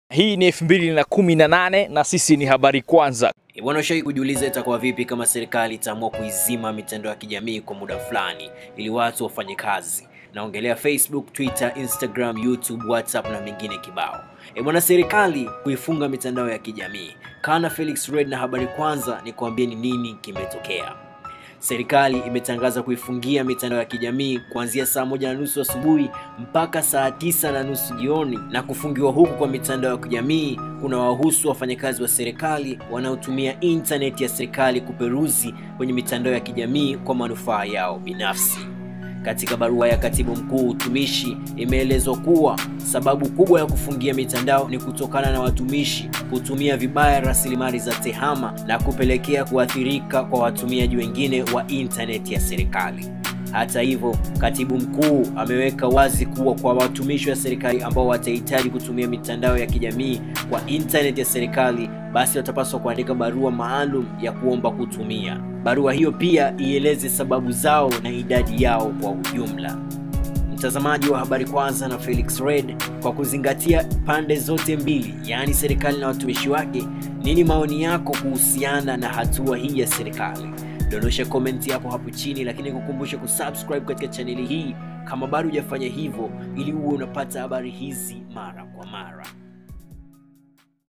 MUHIMU: Serikali Kufungia Mitandao ya Kijamii Muda wa Kazi
Serikali kupitia Katibu Mkuu Utumishi imetangaza kufungia mitandao ya kijamii muda wa kazi kwa wafanyakazi wa serikalini
Kwa Matangazo Wasiliana nasi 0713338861